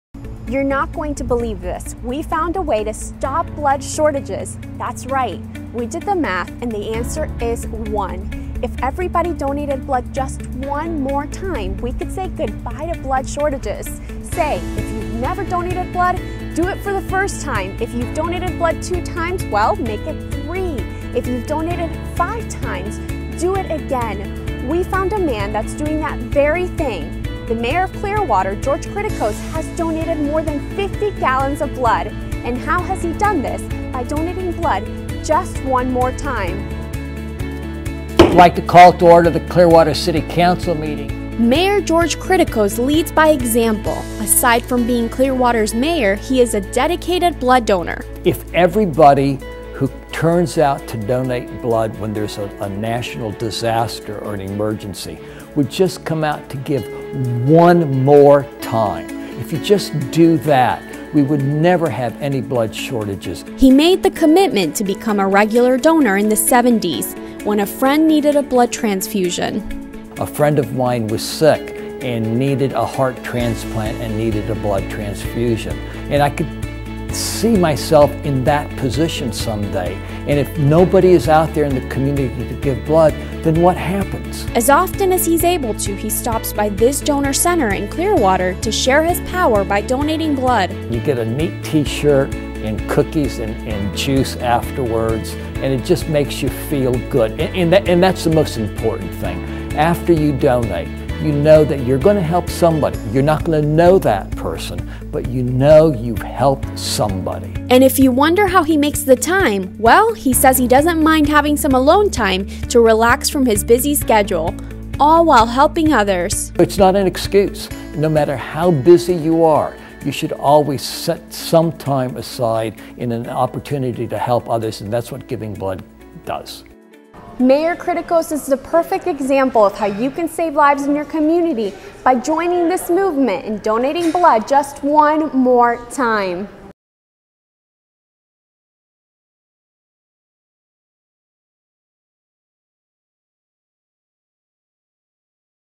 Clearwater, Florida Mayor George Cretekos
George Cretekos leads by example. He has a long history of serving others. For 35 years, George served in both Pinellas County and Washington, D.C. as aide to U.S. Representative C.W. Bill Young. He's also donated over 50 gallons of blood.